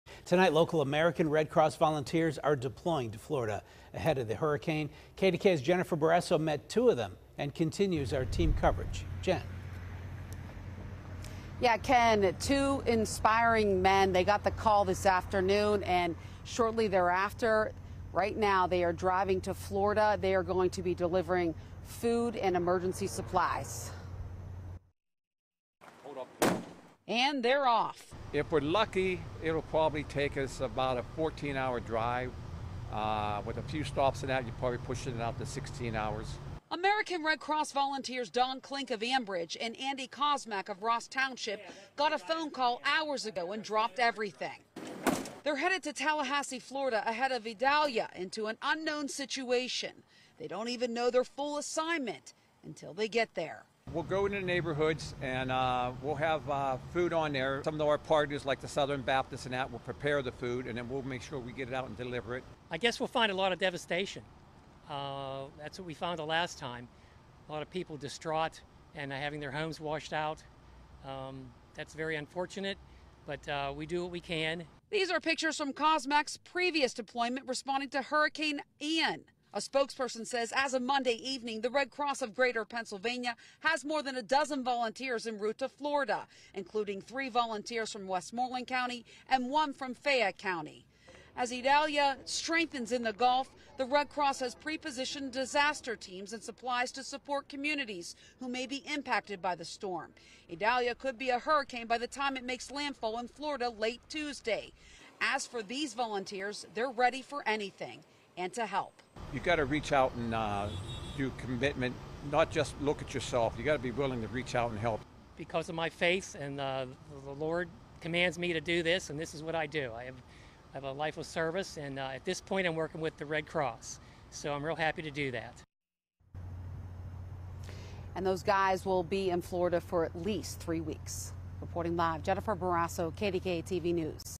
American Red Cross Greater Pennsylvania Region volunteers headed to Florida
More than a dozen volunteers, including six from Western Pennsylvania, are headed to Florida.